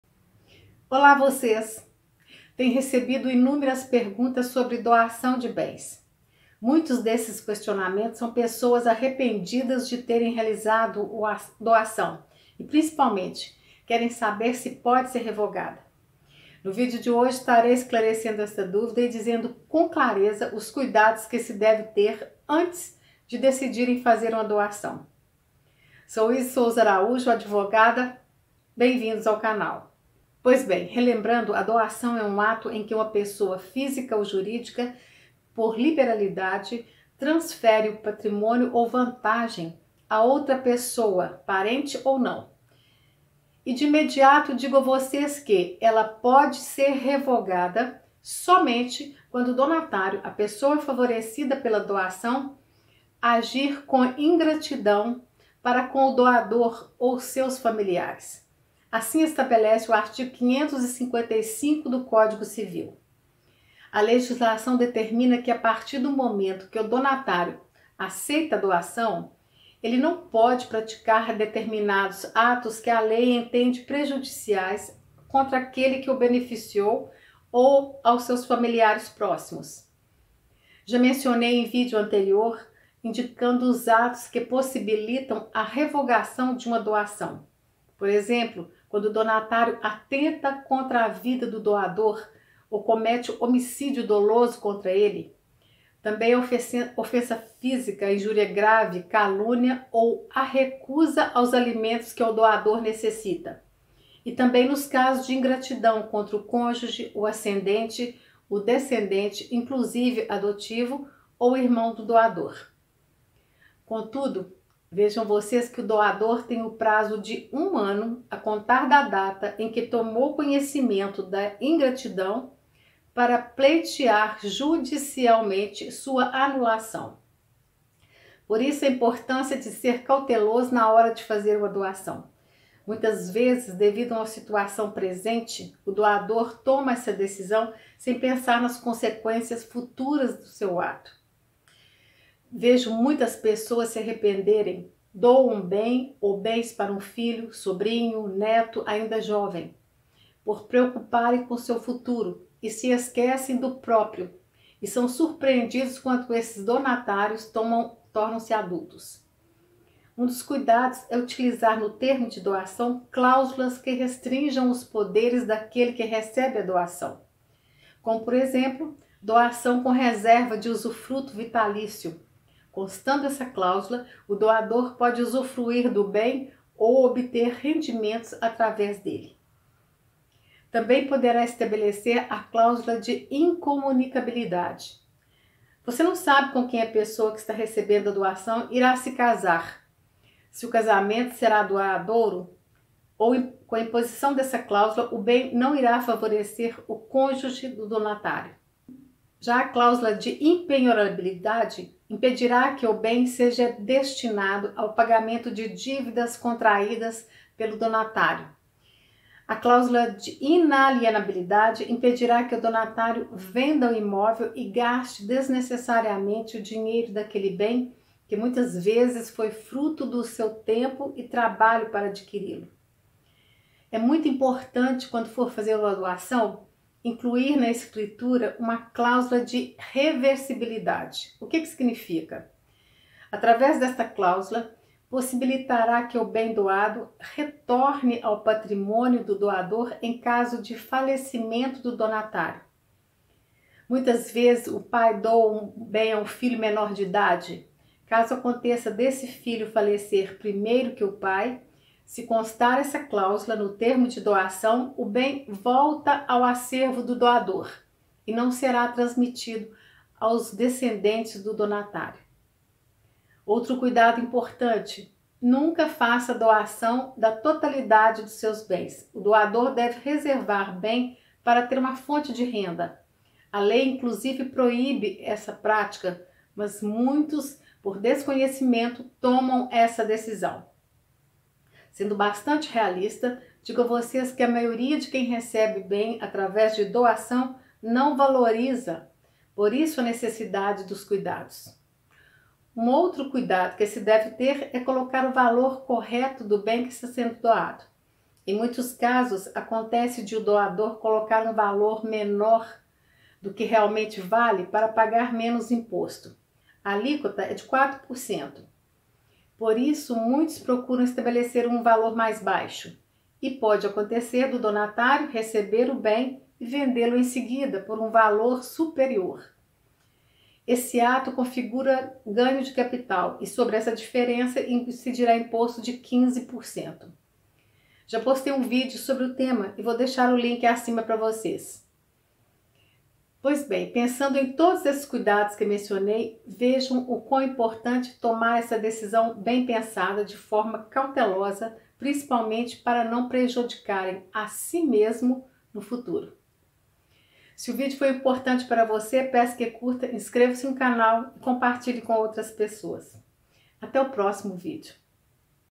CUIDADOS QUE SE DEVE TER ANTES DE FAZER UMA DOAÇÃO
A doação é um ato em que uma pessoa, física ou jurídica, por liberalidade transfere patrimônio ou vantagens à outra pessoa, parente ou não. Muitos doadores com o passar do tempo se arrependem e, principalmente querem saber se pode ser revogada. Segundo estabelece o artigo 555 do Código Civil uma doação pode ser revogada somente quando o donatário (a pessoa favorecido pela doação) agir com ingratidão para com o doador ou seus familiares.
A partir do momento que o donatário aceita a doação, ele não pode praticar determinados atos que a lei entende prejudiciais contra aquele que o beneficiou ou aos seus familiares próximos.
No vídeo esclareço esta dúvida dizendo também com clareza os cuidados que se deve ter antes de decidirem fazer uma doação.